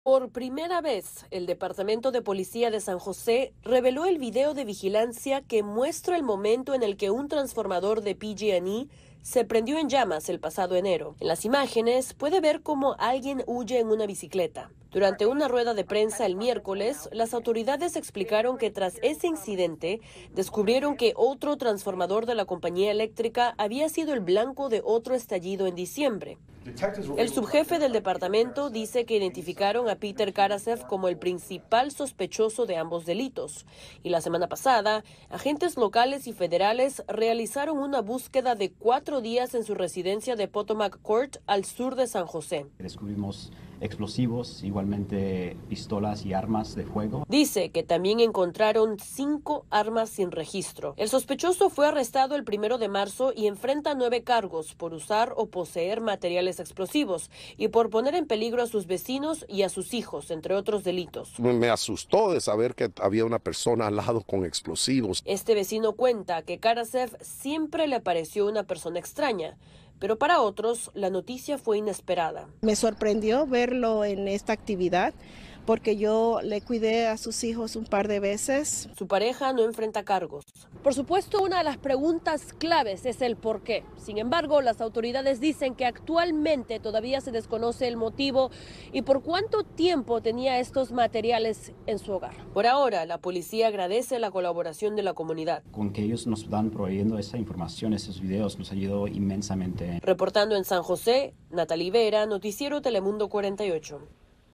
Policía revela detalles tras arresto de sospechoso que tenía explosivos en una casa en San José
Peter Karasev fue arrestado bajo sospechosa de estar involucrado en la explosión de dos transformadores de luz de PG&E.